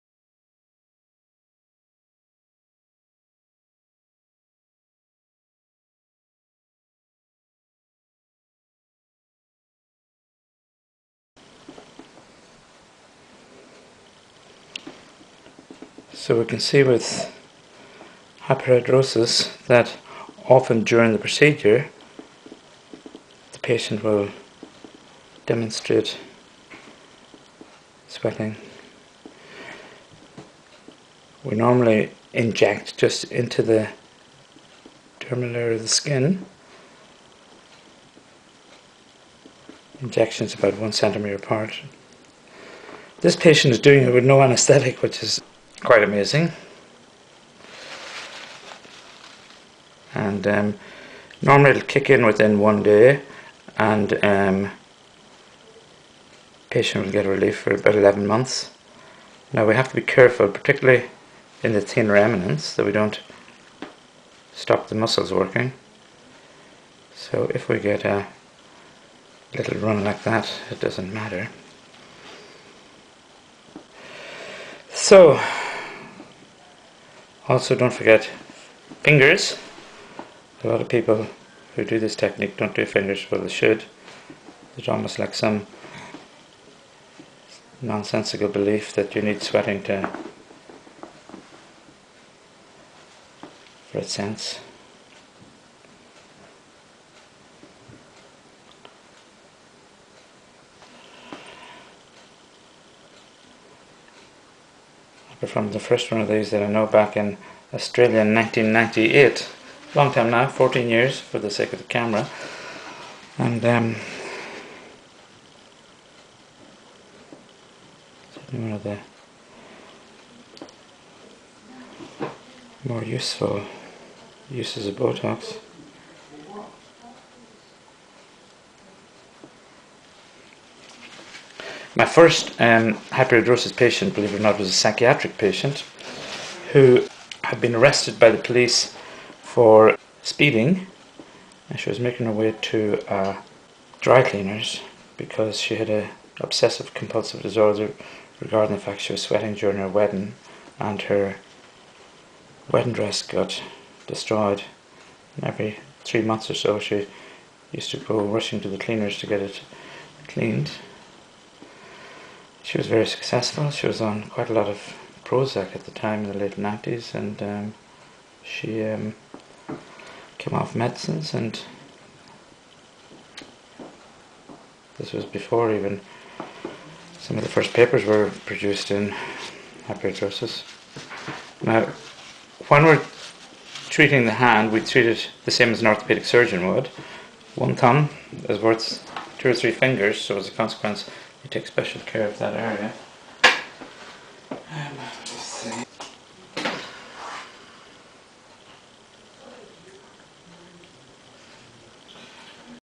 Dr. Patrick Treacy demonstrates Botox for Sweaty Palms
BOTOX® is shown here by Dr. Patrick Treacy for the treatment of primary palmar hyperhidrosis that is inadequately managed with topical agents. This is a very successful treatment which should be performed by an expert practitioner, usually a dermatologist.
The safety and effectiveness of BOTOX® for hyperhidrosis in other body areas have not been established. A weakness of hand muscles and blepharoptosis may occur in patients who receive BOTOX® for palmar hyperhidrosis and facial hyperhidrosis, respectively. Patients should be evaluated for potential causes of secondary hyperhidrosis (e.g., hyperthyroidism) to avoid symptomatic treatment of hyperhidrosis without the diagnosis and/or treatment of the underlying disease. Although effective, the safety and effectiveness of BOTOX® have not been established for the treatment of axillary hyperhidrosis in pediatric patients under age 18.